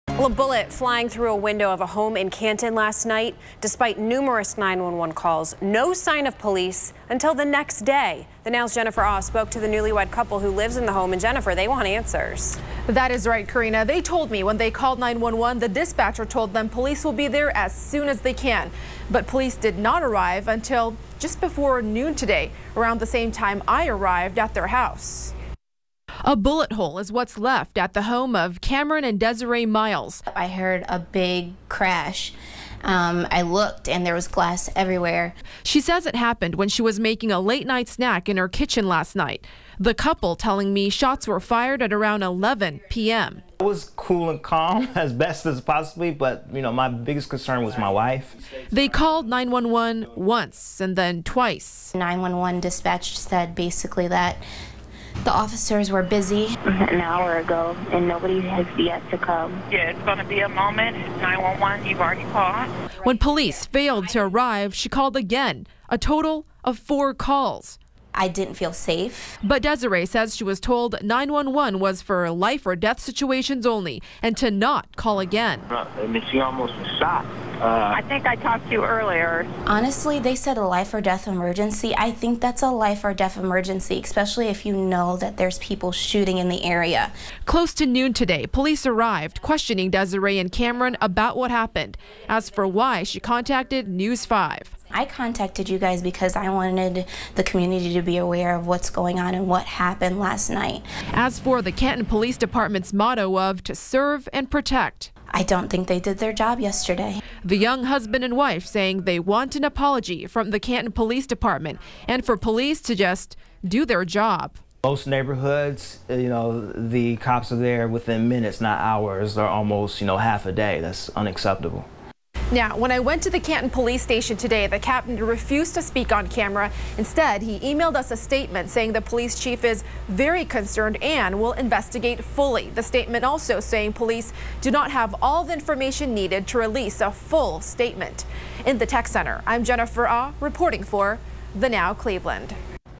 Bullet flies through window of Canton home
Bullet flies through window of Canton home -- Jennifer Auh reports